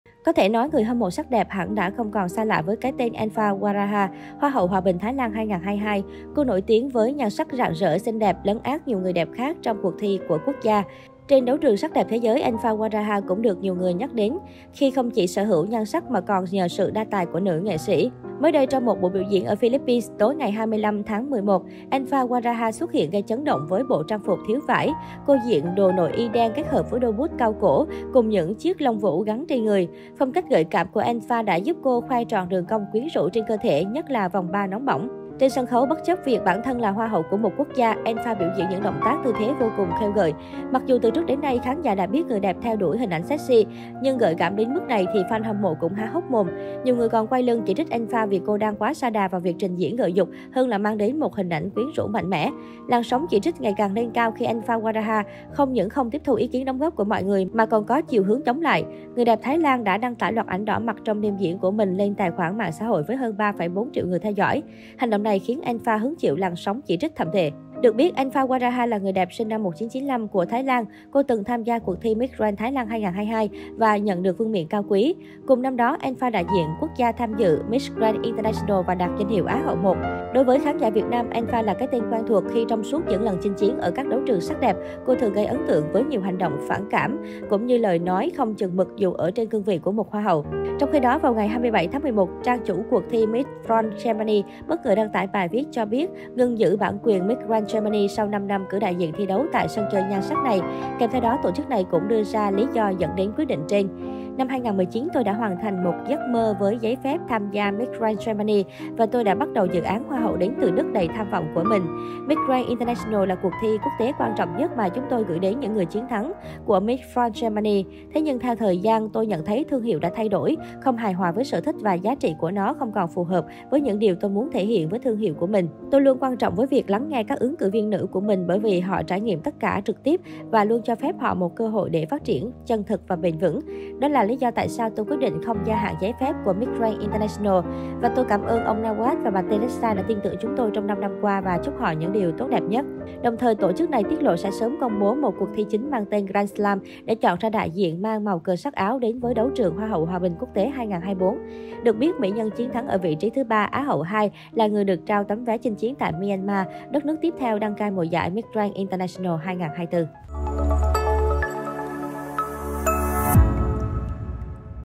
Engfa như không mặc đồ, diễn cảnh phòng the trên sân khấu, Miss Grand “muối mặt” nhận tin dữ
Có thể nói, người hâm mộ sắc đẹp hẳn sẽ không còn xa lạ với cái tên Engfa Wahara - Hoa hậu Hòa Bình Thái Lan 2022. Cô nổi tiếng với nhan sắc rạng rỡ, xinh đẹp, lấn át nhiều người đẹp khác trong cuộc thi của quốc gia. Trên đấu trường sắc đẹp thế giới, Engfa Wahara cũng được nhiều người nhắc đến không chỉ vì nhan sắc của cô mà còn nhờ sự đa tài của nữ nghệ sĩ.

Mới đây, trong một buổi biểu diễn ở Philippines tối ngày 25/11,Engfa Wahara xuất hiện gây chấn động với bộ trang phục thiếu vải. Cô diện đồ nội y đen kết hợp với đôi boots cao cổ cùng những chiếc lông vũ gắn trên người. Phong cách gợi cảm của Engfa đã giúp cô khoe trọn đường cong quyến rũ trên cơ thể, nhất là vòng 3 nóng bỏng.